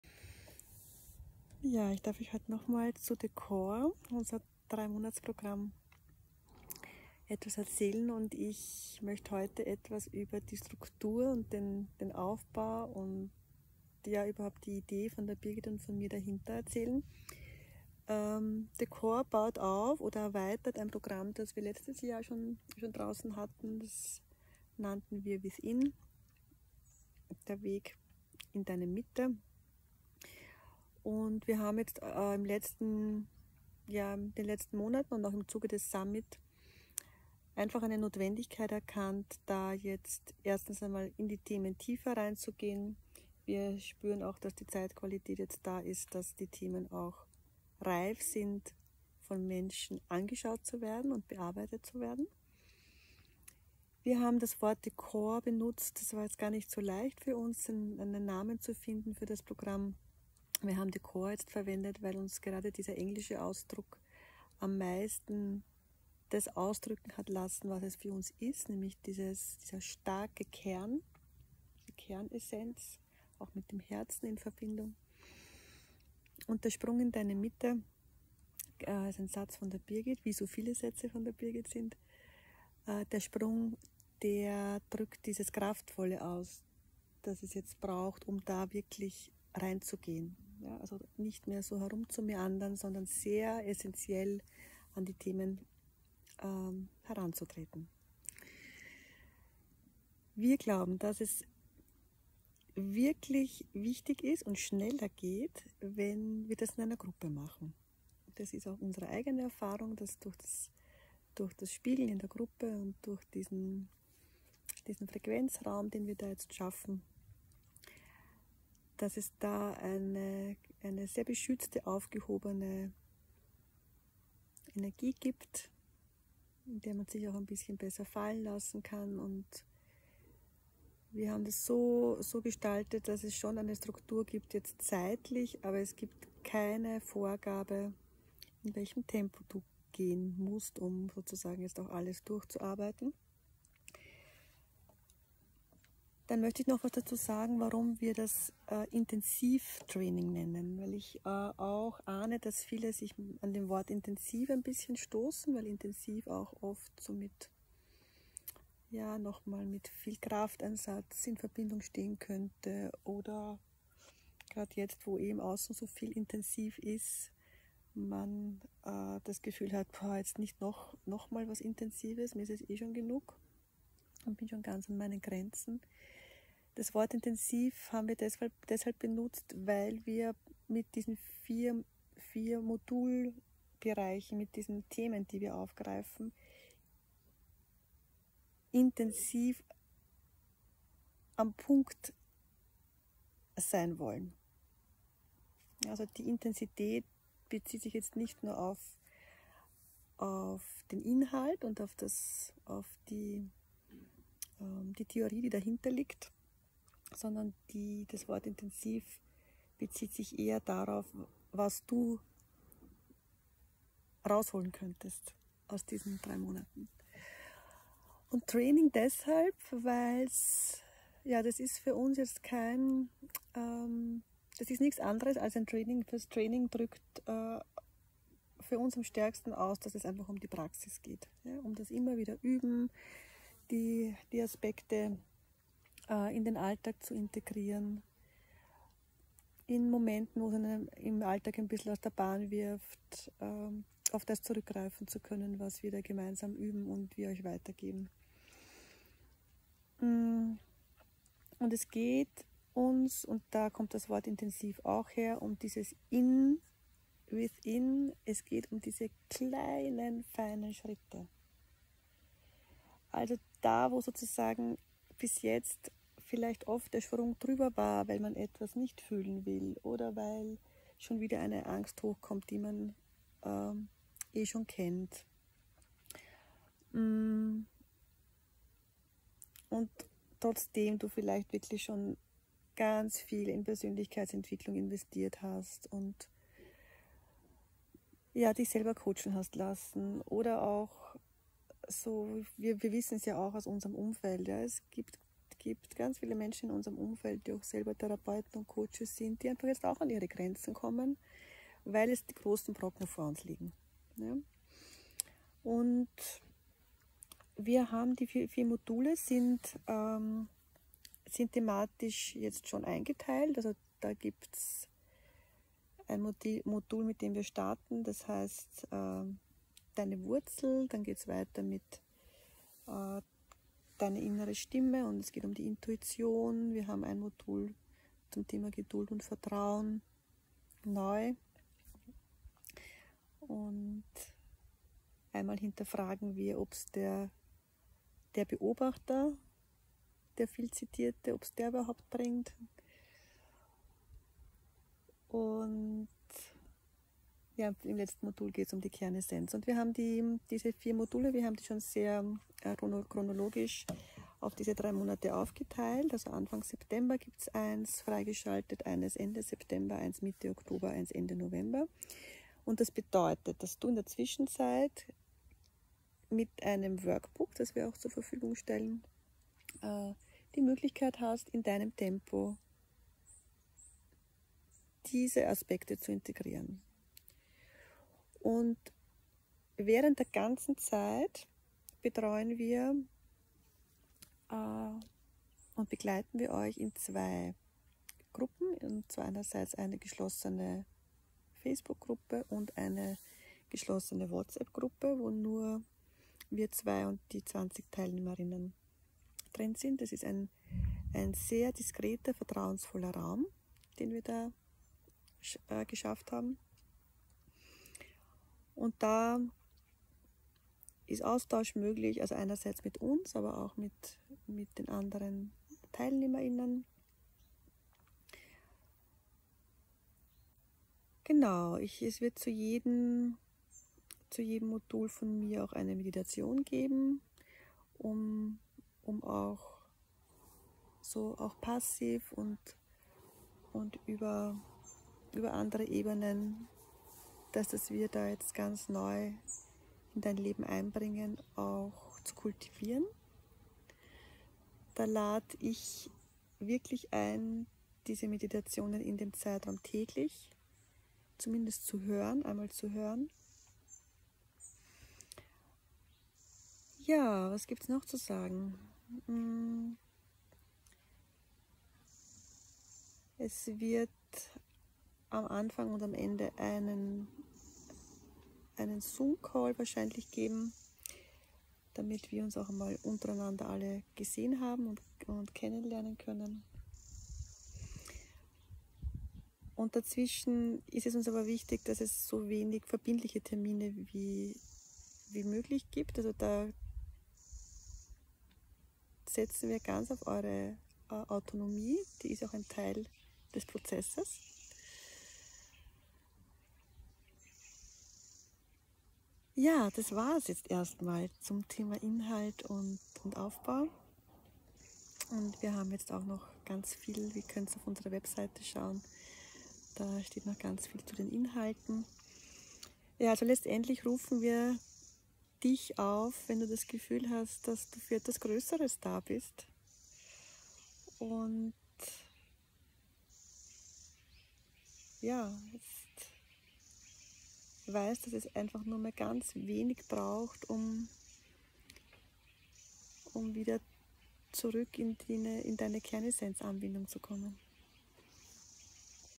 THE CORE - Video #4 von Alexandra
🌟About THE CORE / Video #4☀️

🌟THE CORE. Der Sprung in Deine Mitte‘
In diesem Video spreche ich über den Ablauf, die Inhalte, unsere Vision und warum wir bewusst diese Worte u Sätze benutzen:
👍THE CORE
🫶 Der SPRUNG in deine Mitte
👍 die Kernessenz
🫶 intensiv
👍 Training

Warum wir hier das Gruppensetting wählen.
🧩Birgit & ich stehen in dem Zeitraum auch für 1:1 zur Verfügung. Bitte individuell direkt bei uns anfragen 🧩
Wir geben das, was bei uns im Moment überfließt an Kraft, Bewegung, Anbindung und Erfahrungsreichtum.

🧩No show
🧩Keine Versprechen

🌊 es geht nur mit deinem inneren JA
🌊 wir rufen die, die richtig Lust auf Veränderung haben - aus der Kraft ihres Herzens
🌊 du fühlst es, wenn’s passt

🌈Es geht nach innen.
In die Zwischenräume
Dort und nur dort wird’s leichter.
DURCHATMEN
Anmeldung geöffnet/
20 Plätze
3 Monate
4 Module
Geschützte Räume
Und danach: spread it! In deinem Umfeld ⭐️🌈

€ 1.200,-

Nächster 3Monats Durchgang erst wieder ab April 2024